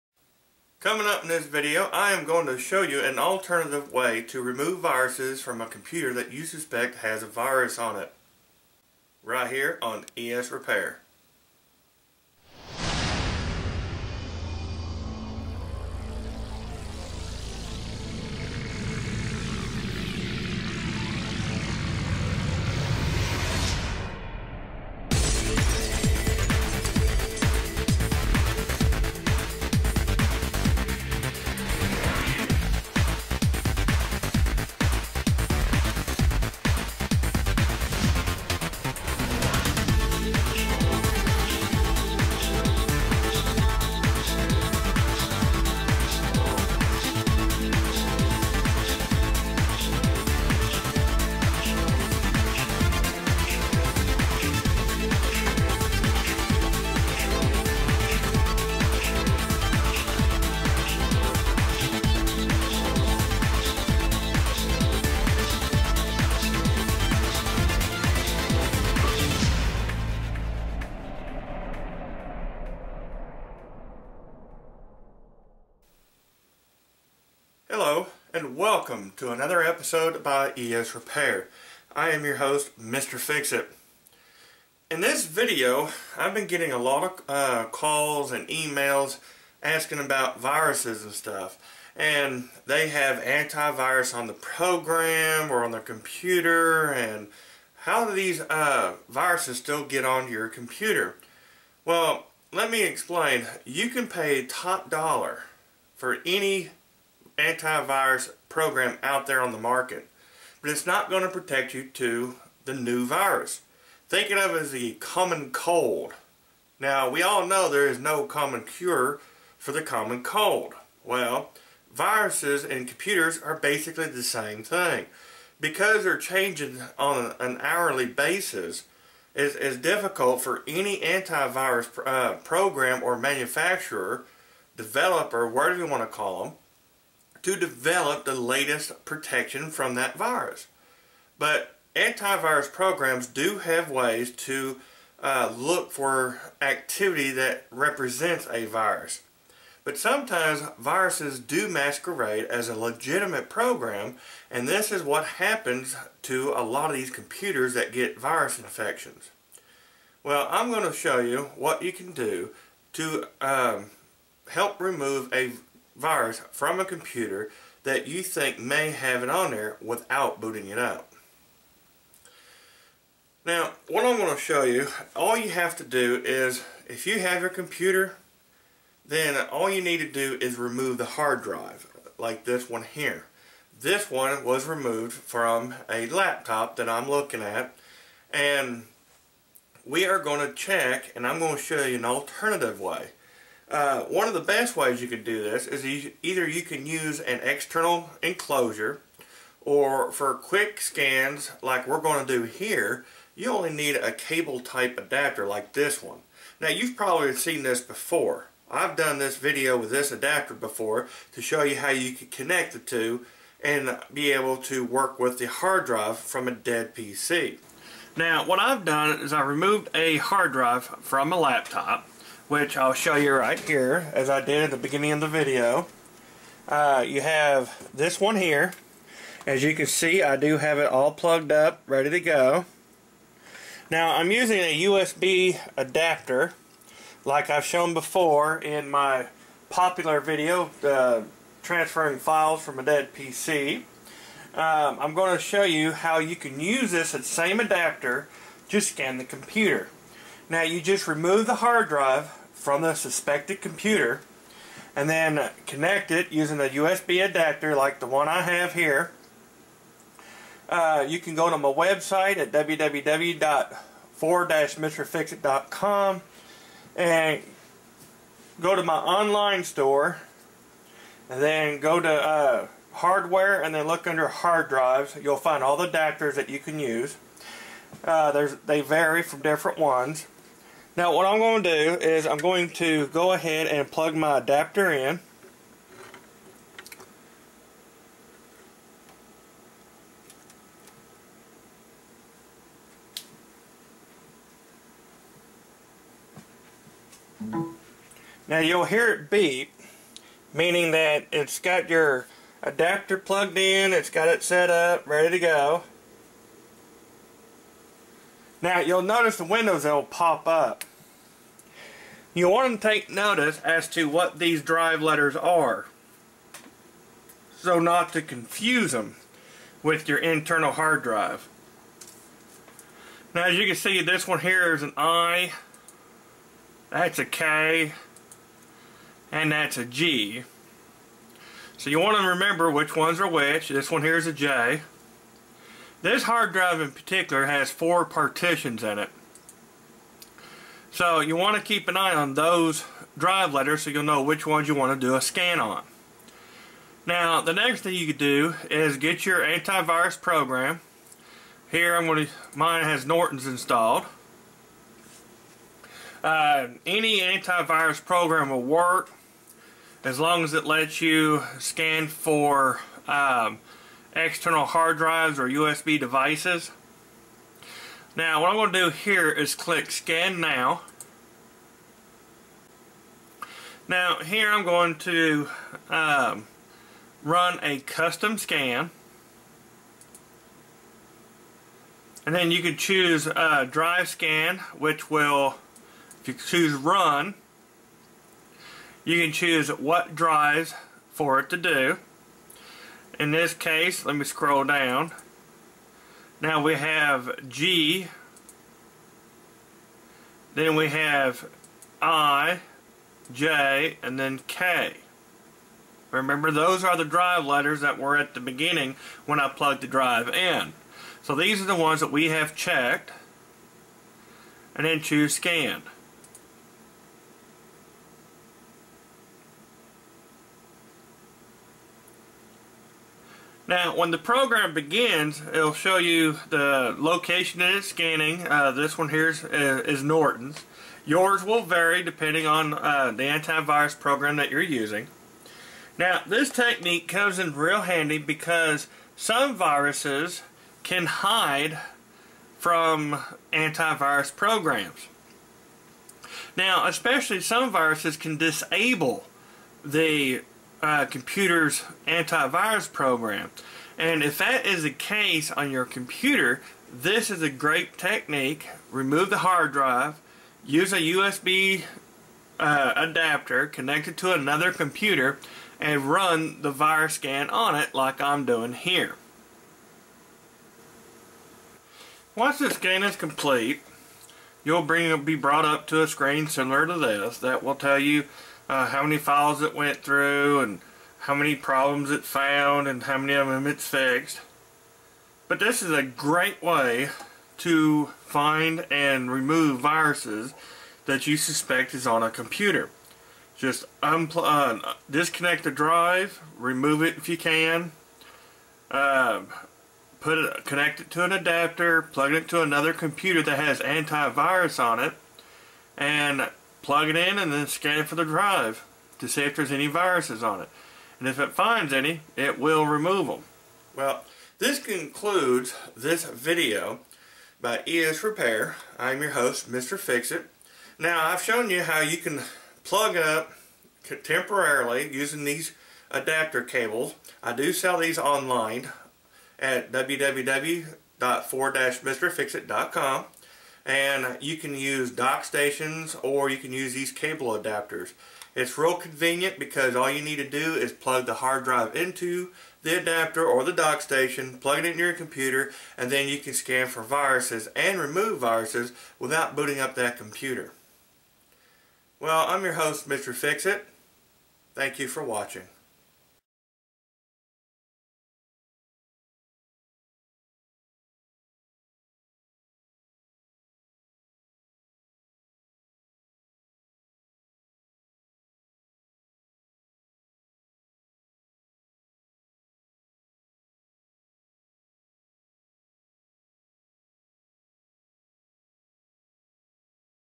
How to safely remove viruses from an infected PC
Once a Virus infects a PC, it can be difficult to remove the virus while the OS is operating. I will show a safe method that can remove viruses on an infected PC without booting it's Operating System. Need an adapter or an anti-virus program?